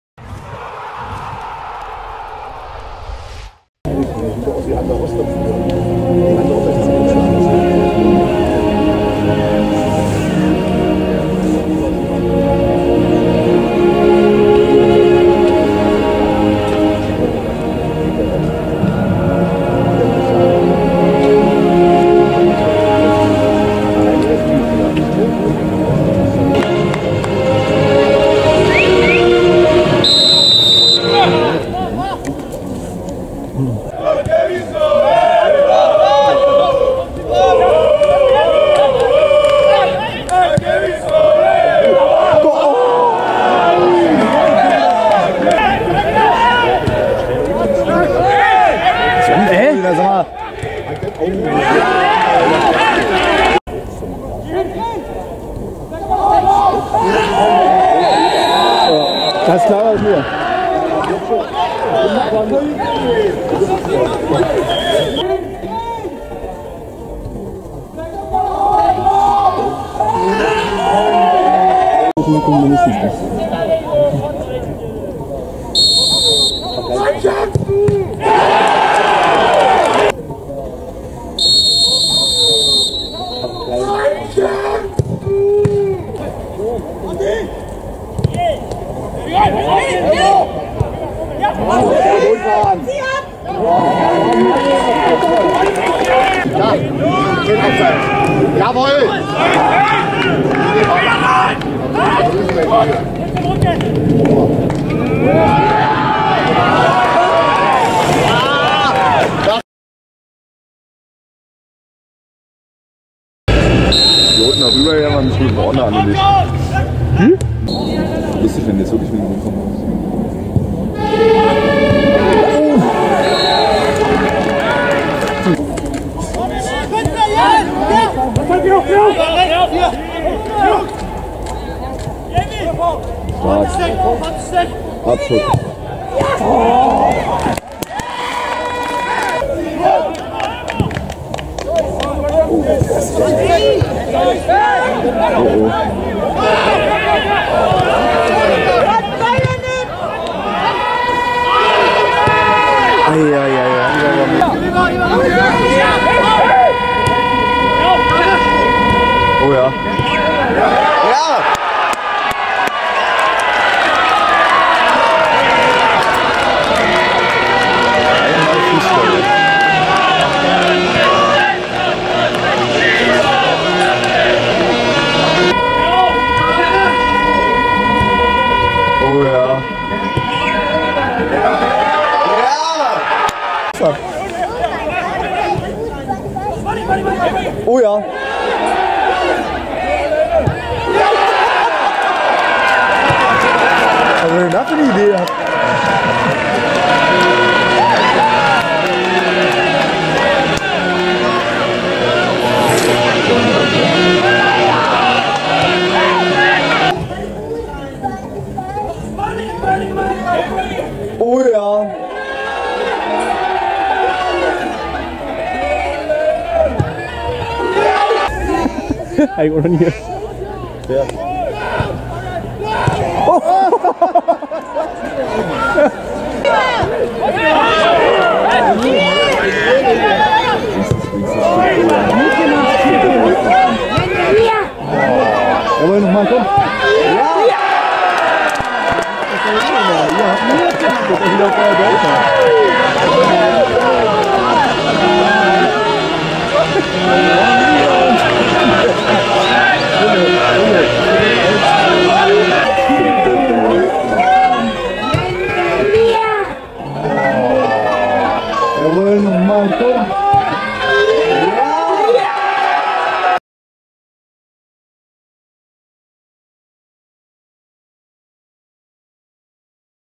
9. Spieltag - 22.10.2016 - Lichtenberg 47 - FC Anker Wismar - Spielszenen 47er TV
Am 9. Spieltag empfingen unsere Farben die Gäste aus Wismar und konnten diese, dank einer effizienten Torausbeute im Durchgang zwei, mit einem verdienten und deutlichen 3:0 (0:0) nach Hause schicken. Die Tore schossen Thomas Brechler (77'), Philip Einsiedler (84') und Christian Gawe in der Nachspielzeit.